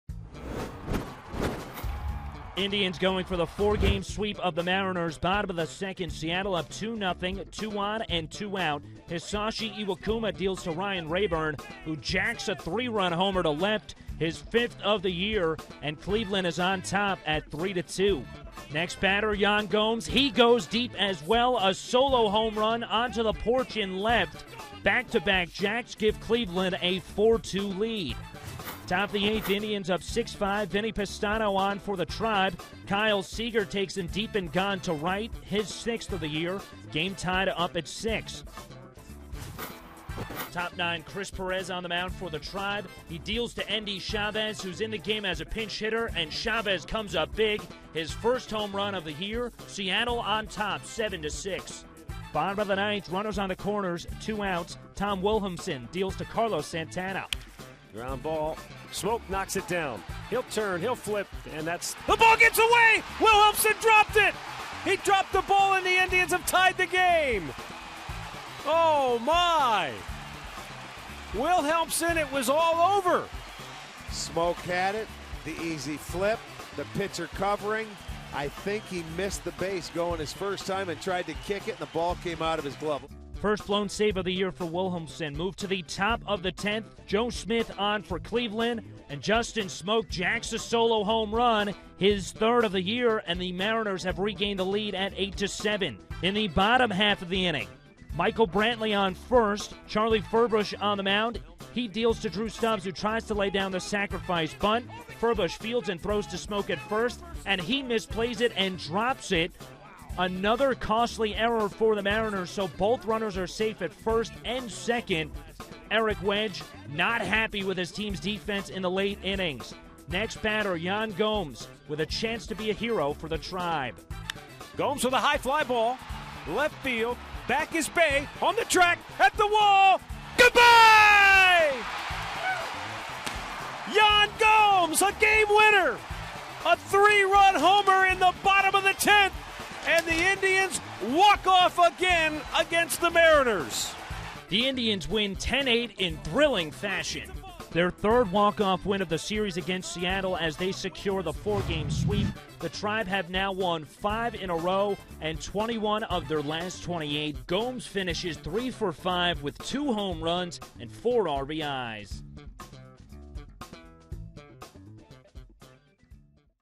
2013/05/20 Recap: SEA 8, CLE 10 F/10
5/20/13: Gomes' walk-off homer lifts Tribe in extras